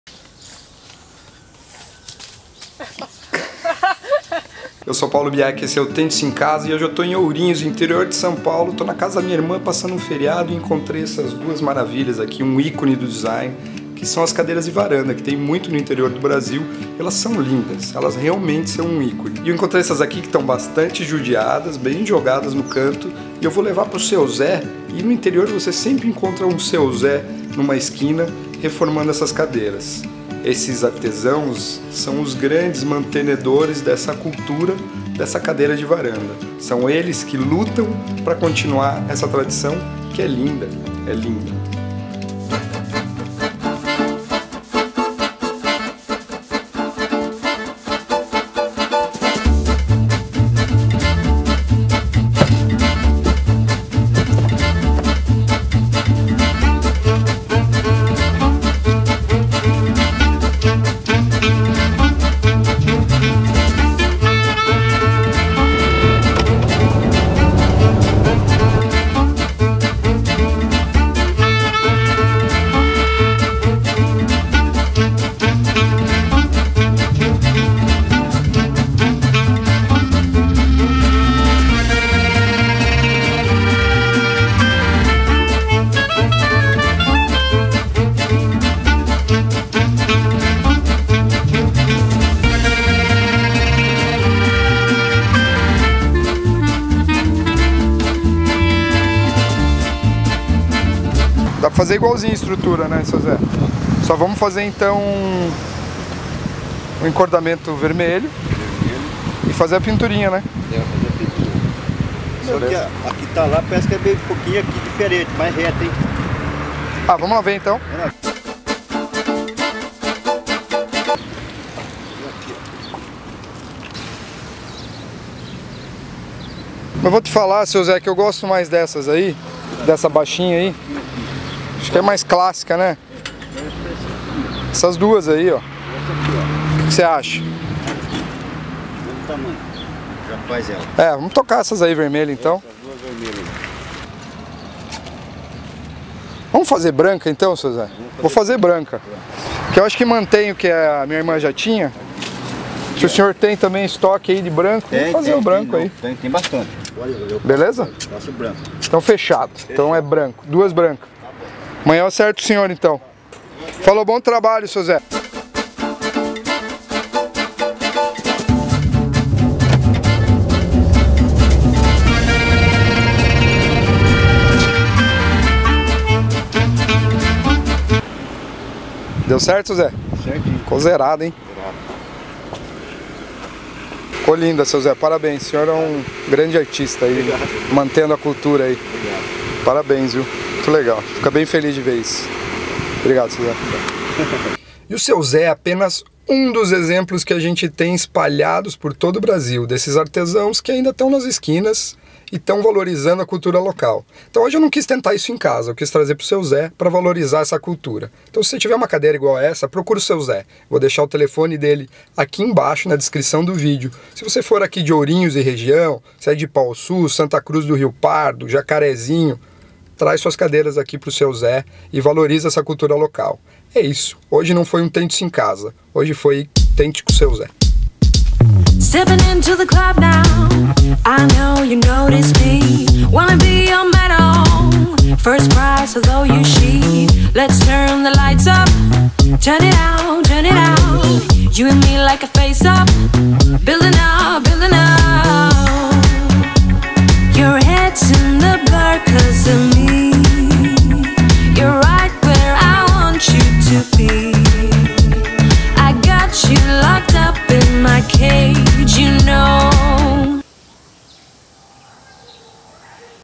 COMO REFORMAR POLTRONA CADEIRA ESPAGUETE | DESIGN | PAULO BIACCHI
A poltrona espaguete é um clássico do interior do Brasil, fui pra Ourinhos e veja no que deu!

Seu Zé (Ourinhos) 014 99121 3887

Ideia Original e Apresentação: Paulo Biacchi
Produção Executiva: Carolina Armellini